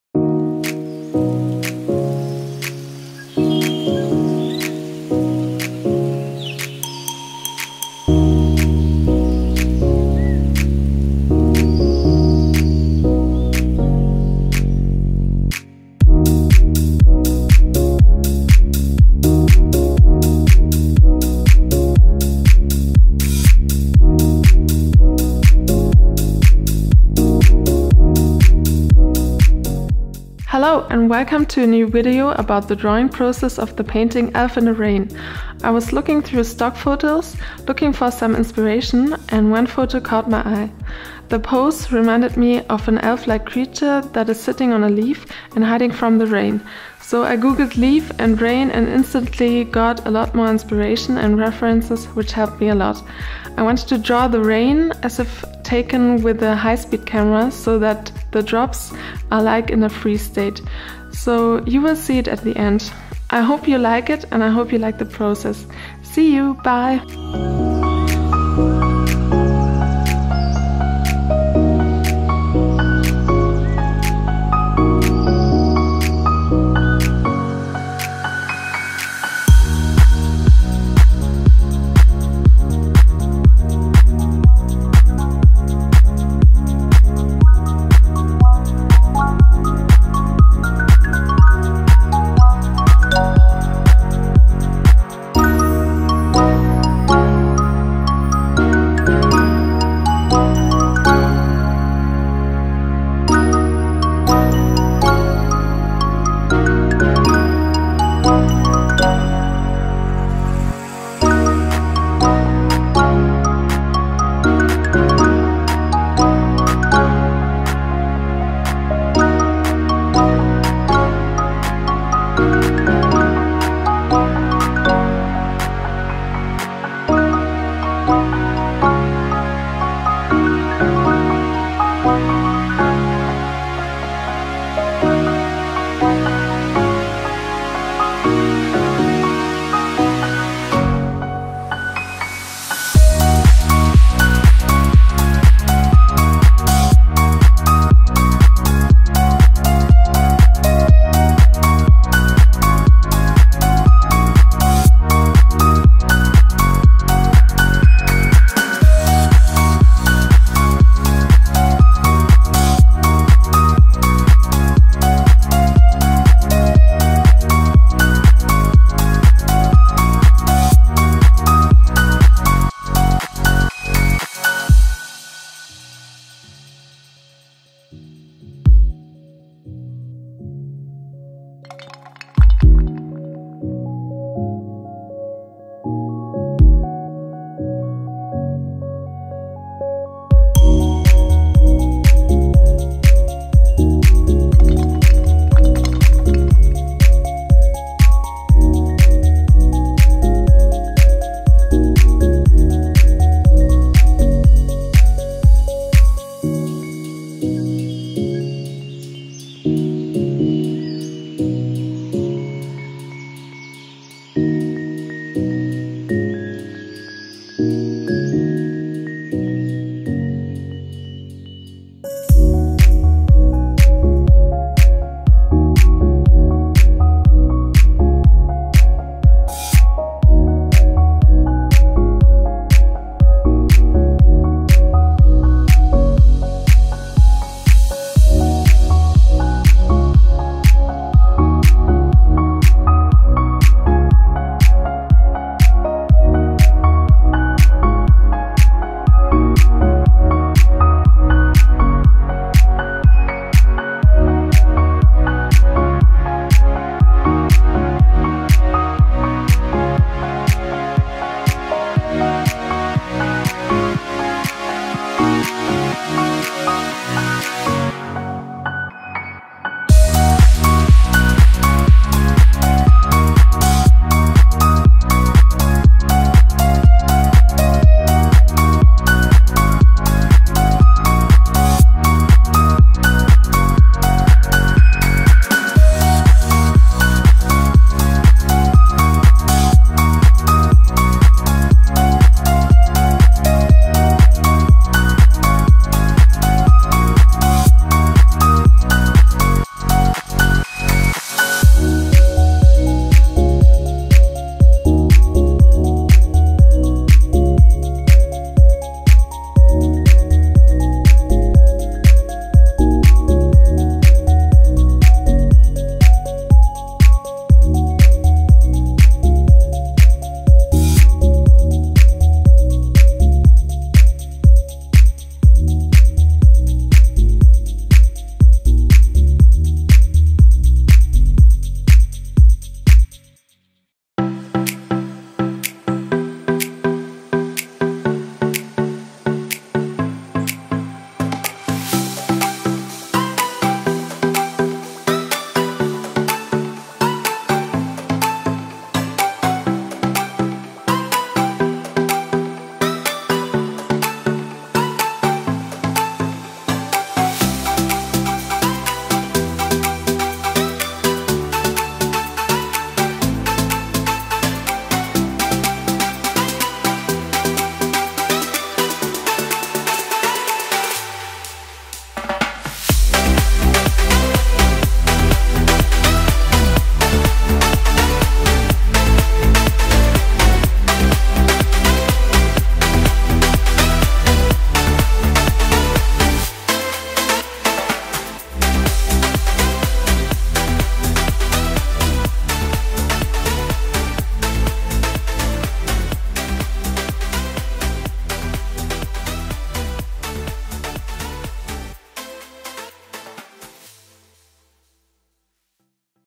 Elf in the Rain - Drawing Process (speeded up)
Glad to be finished with this piece. I like everything that's fantasy themed so it was time to draw something fairy or elf-like...